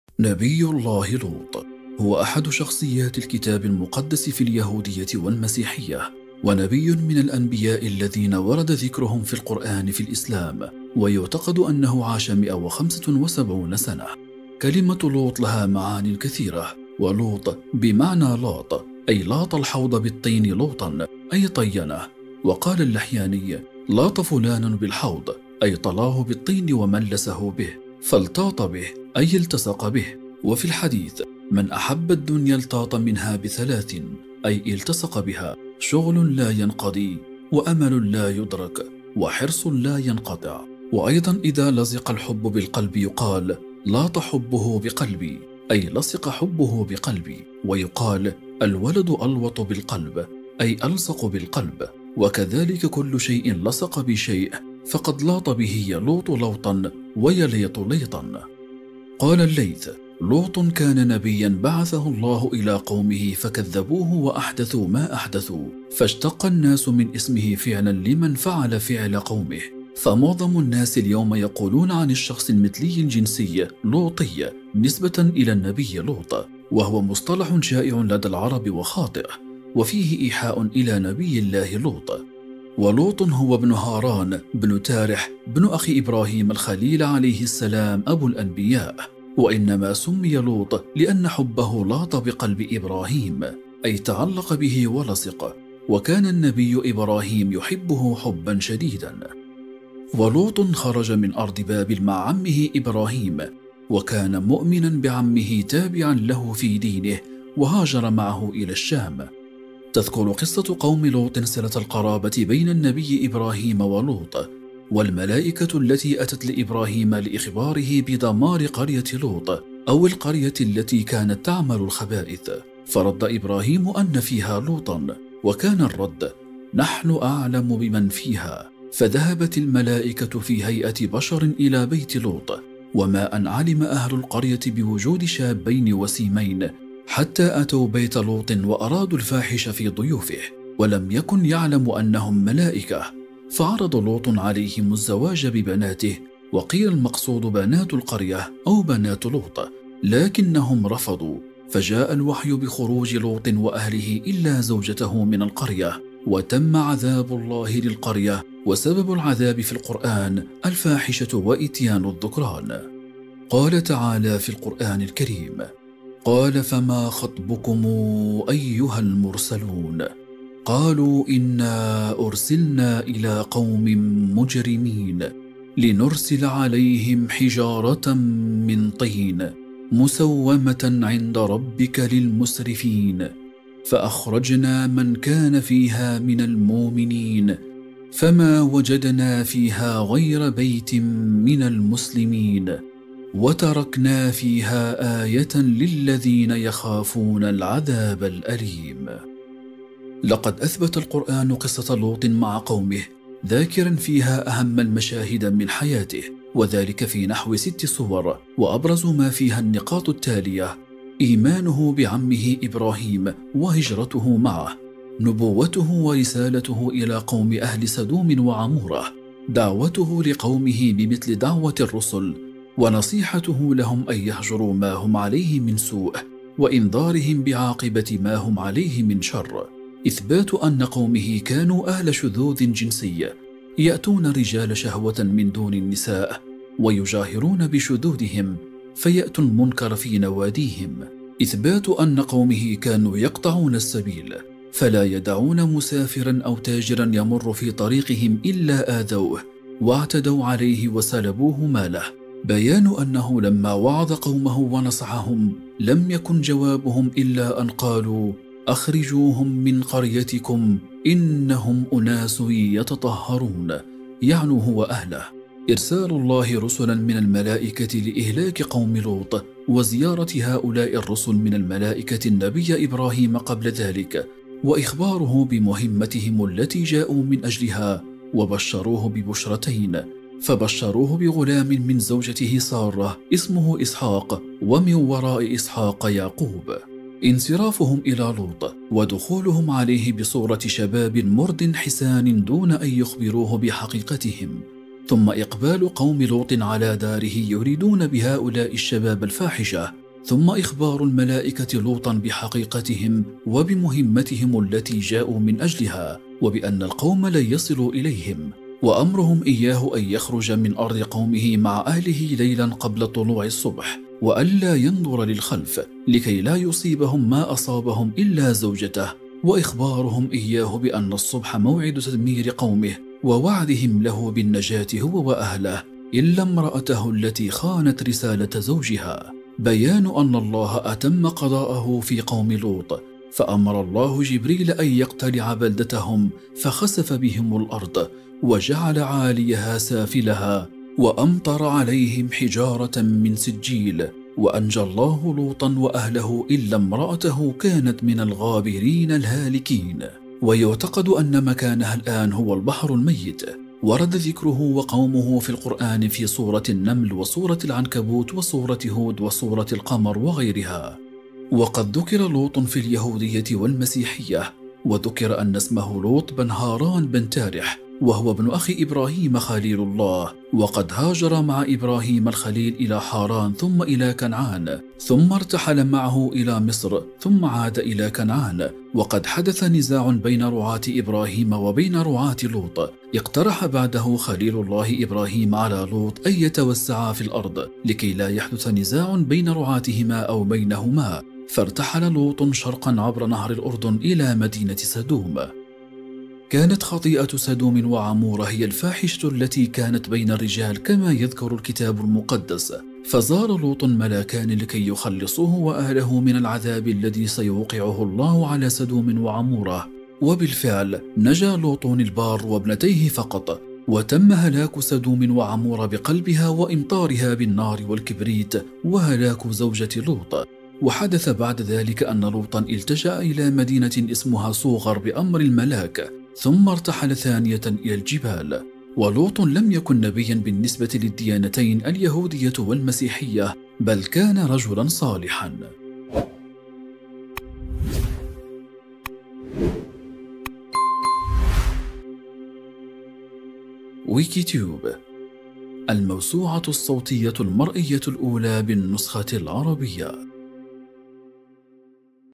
لوط نبي الله
نبي الله لوط بن حاران بن تارح وهو ابن اخ ابراهيم الخليل و ملازمه في سفره الى فلسطين